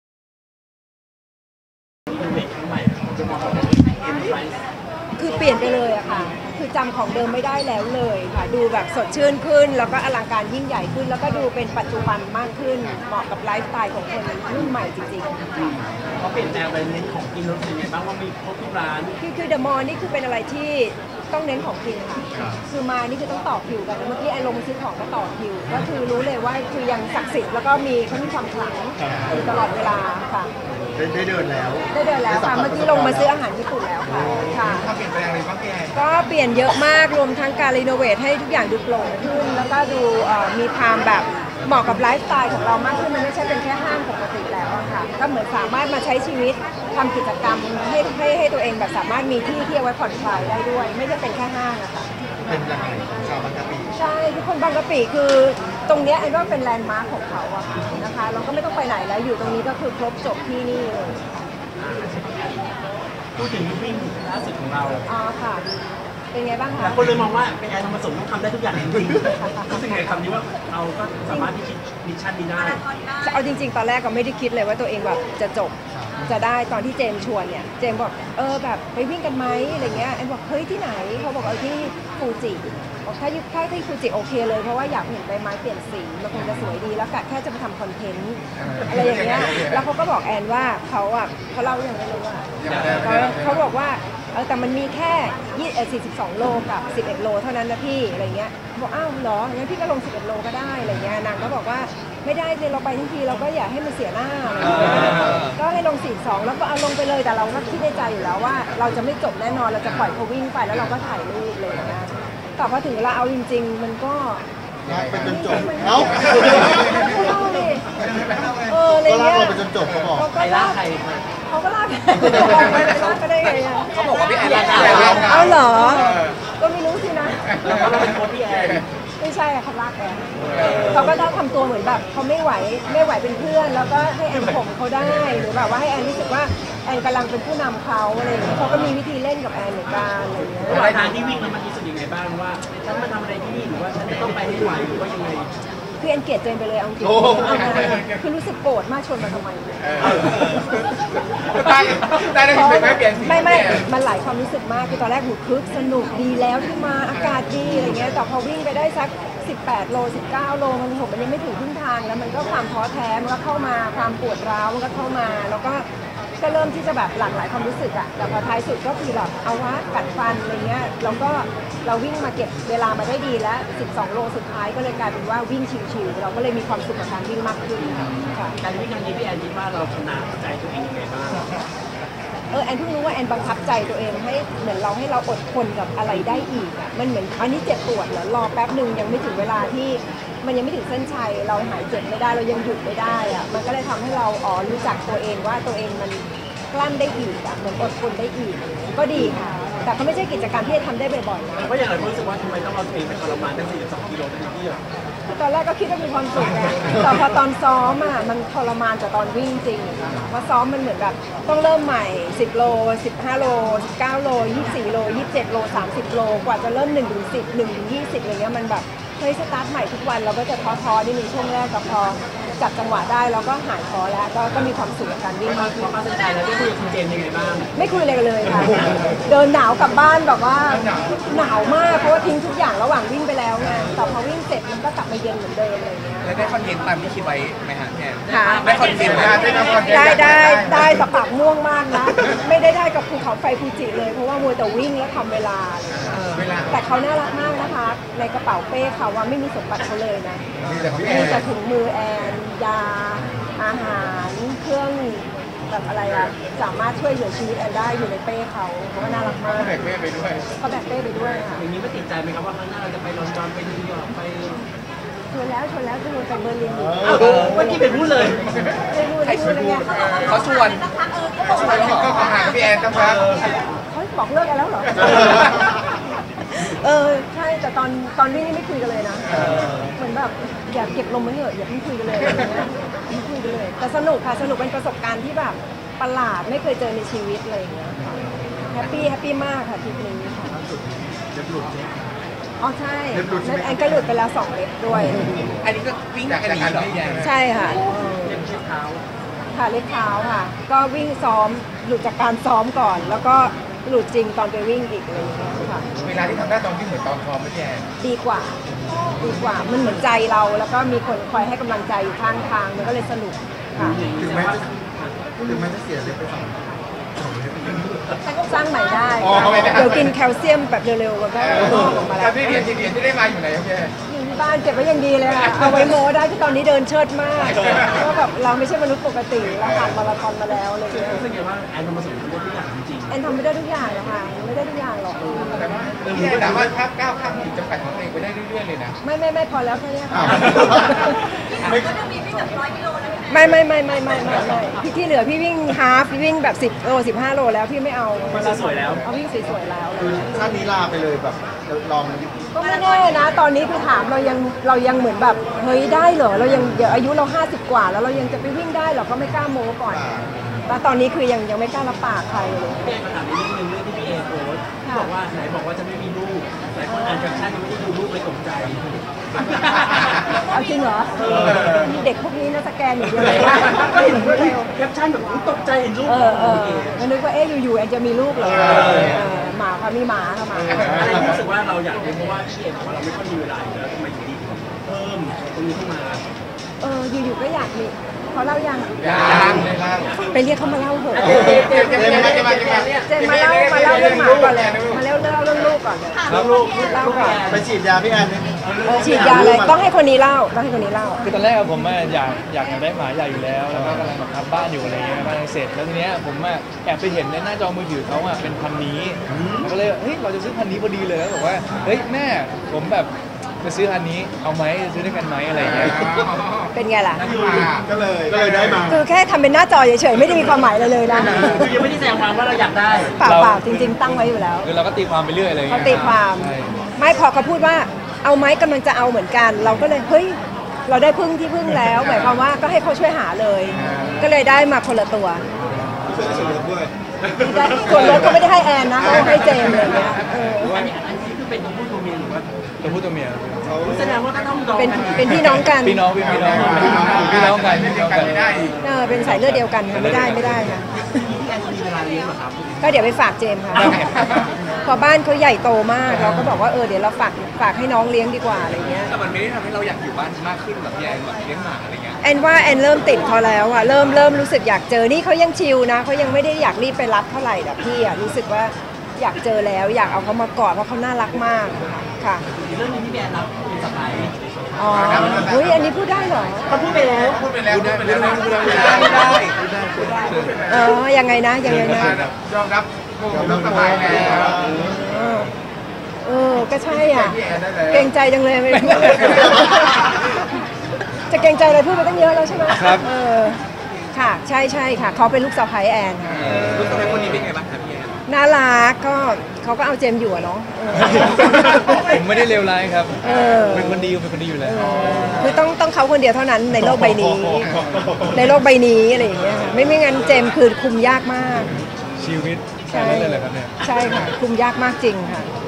แอน โกรธ เจมส์จิ ชวนไปวิ่งมาราธอนทำไม เรียกเจมส์จิมาเล่าชวนแอนซื้อหมา ได้มาคนละตัว
แอน ทองประสม เผยกับการวิ่งมาราธอน 42 กิโลเมตร จริงๆไม่คิดว่าจะจบ แต่พอจริงๆก็ลากกันไปจนจบ ความรู้สึกตอนนั้นโกรธเจมส์เลย ชวนไปทำไม มันหลายความรู้สึกมาก วิ่งแรกๆก็สนุกมาก แต่พอไปเริ่มปวดร้าว เลยกัดฟัน แต่หลังๆพอเก็บเวลาดีแล้วก็วิ่งชิวๆเลยกลับมามีความสุข วิ่งเสร็จก็ไม่คุยกับเจมส์เลย ชมอีกฝ่ายน่ารักในกระเป๋าเป้มีแต่ของแอนทั้งนั้นเลย ถือเป็นประสบการณ์ที่ไม่เคยเจอในชีวิต เล่าทั้งซ้อมและจริงทำเล็บหลุดด้วย ลั่นแอนทำไม่ได้ทุกอย่างหรอกหลังคนบอก แอน ทองประสม ทำได้ทุกอย่าง แจงพี่เอโพสต์แคปชั่น ไหนจะไม่มีลูก แล้วคนตกใจ ถ้าไปดูรูปคือเลี้ยงหมา เรียกเจมส์มาเล่าเป็นคนชวนพี่แอนเพราะตนจะซื้อแล้วเห็นพี่แอนตั้งรูปพันธ์ุนี้ที่หน้าจอ สรุปได้มาคนละตัว เป็นพี่น้องกัน แอนบอกจะได้ไว้ฝากเจมส์เลี้ยง รับเริ่มติดน้องแล้วอยากเจอ ถือโอกาสชมย้ำบอกโฟมเขาเป็นลูกสะใภ้แอน ชมเขาเป็นคนน่ารัก เอาเจมส์อยู่